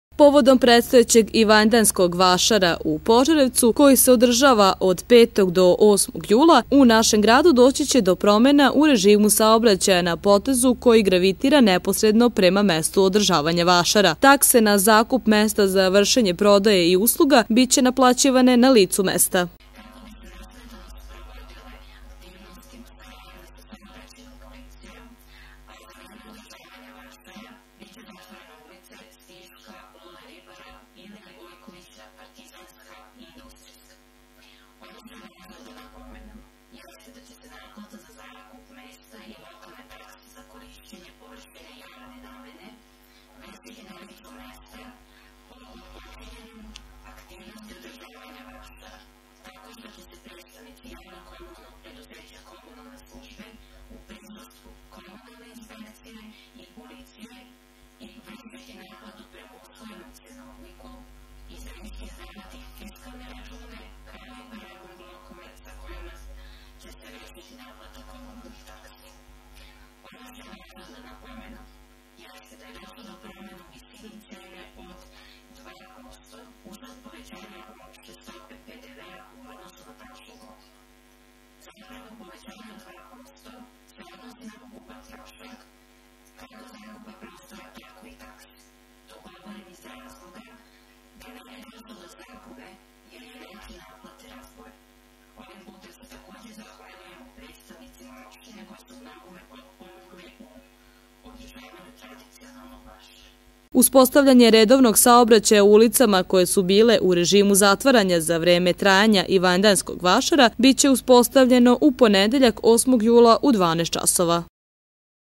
Promenjen režim saobraćaja za vreme trajanja Ivanjdanskog vašara, 05.07.2013.
Za vreme trajanja tradicionalnog vašara u Požarevcu doći će do promene režima saobraćaja. O ovome, kao i o naplati vašarskih taksi, za našu televiziju govori Vesna Todorović, zamenik direktora JKP "Komunalne službe".